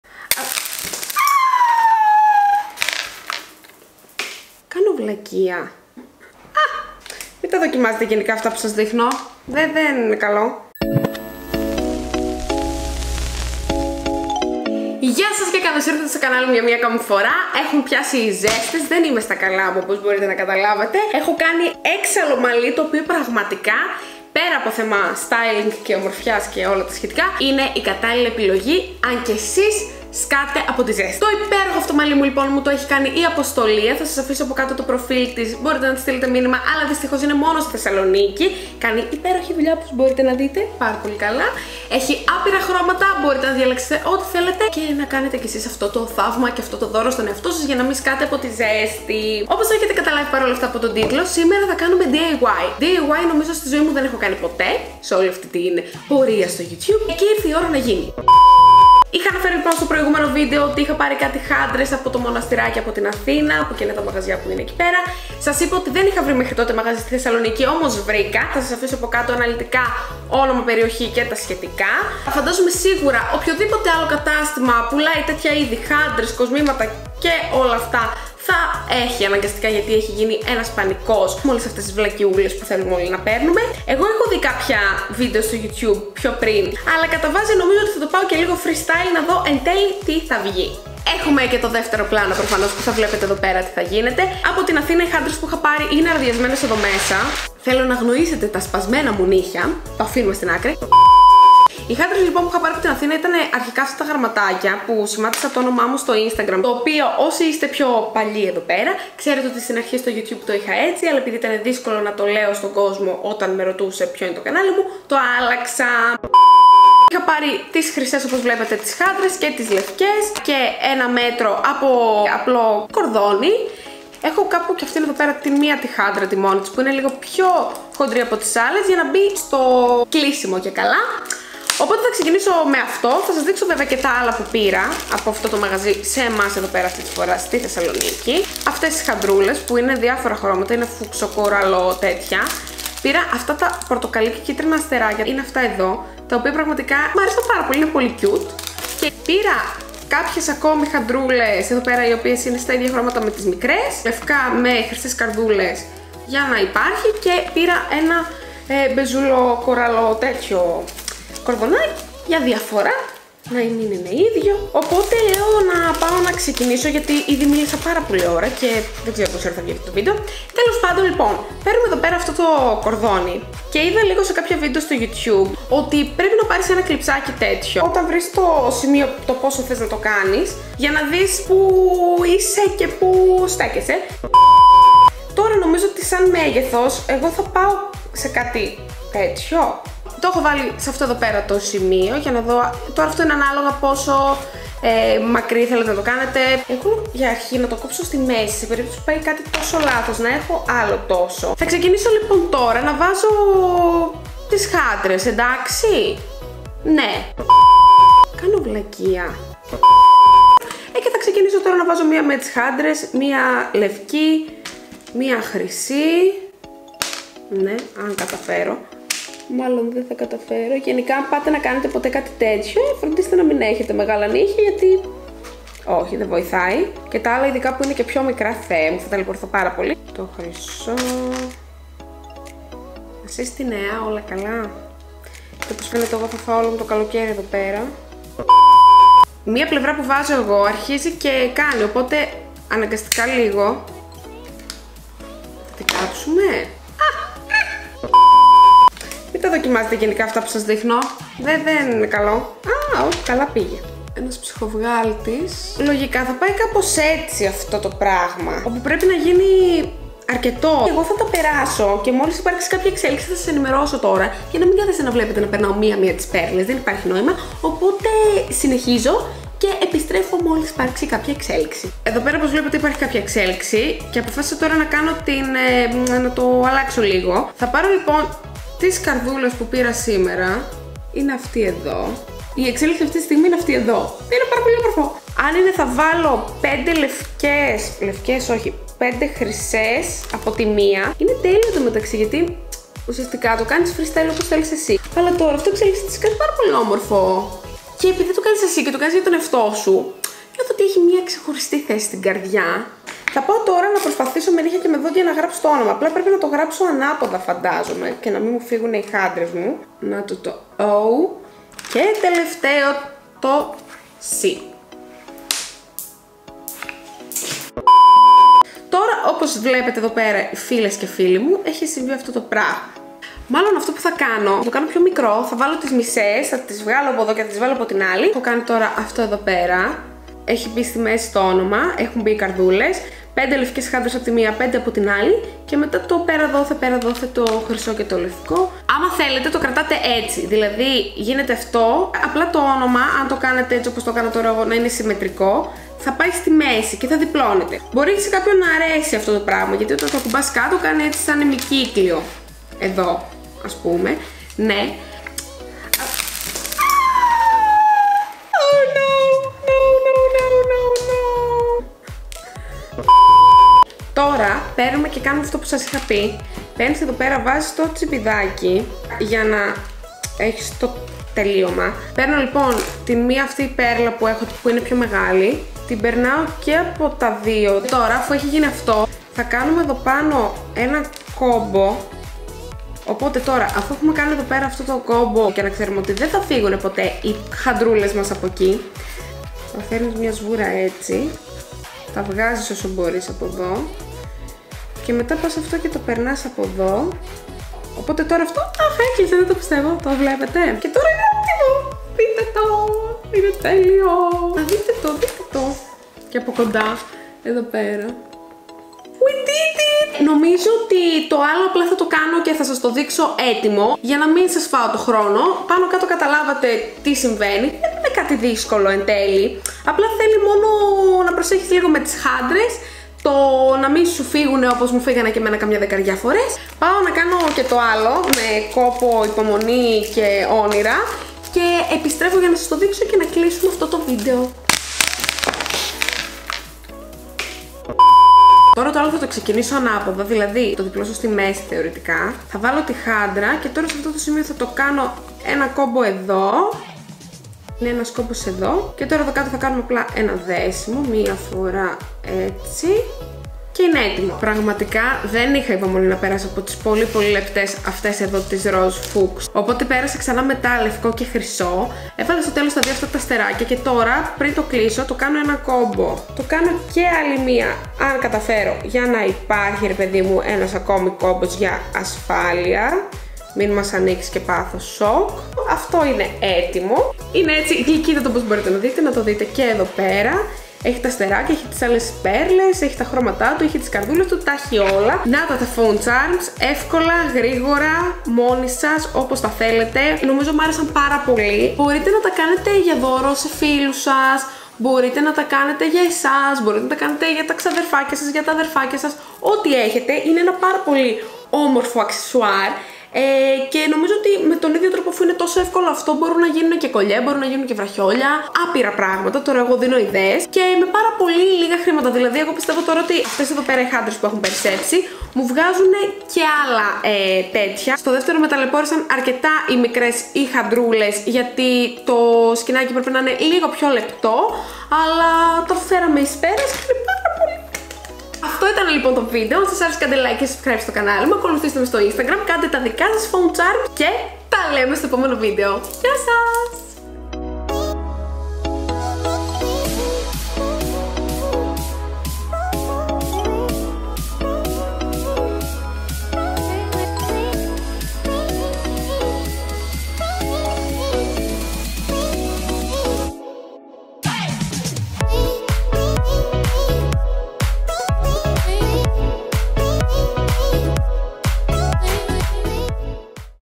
DIY: Phone Charms , εύκολα & απλά | Katerina Konstantinidou
DIY πρώτη φορά στην ιστορία του καναλιού!😂 Αν θέλεις και εσύ να κάνεις τα δικά σου Phone Charms τότε βρήκες τον άνθρωπο σου. Εύκολα , γρήγορα & απλά άσχετα που εγώ ταλαιπωρήθηκα λίγο γιατί δεν έχω και υπομονή.

Τα μαλλιά μου τα έχει περιποιηθεί η @apostolia_tsoukalaa ❤️

17 ΝΟΕΜΒΡΗ 55 ΤΚ 55534
ΚΩΝΣΤΑΝΤΙΝΟΠΟΛΙΤΙΚΑ ΘΕΣΣΑΛΟΝΙΚΗ

►Filmed & Edited by Katerina Konstantinidou

►Editing Software : iMovie & Premiere Pro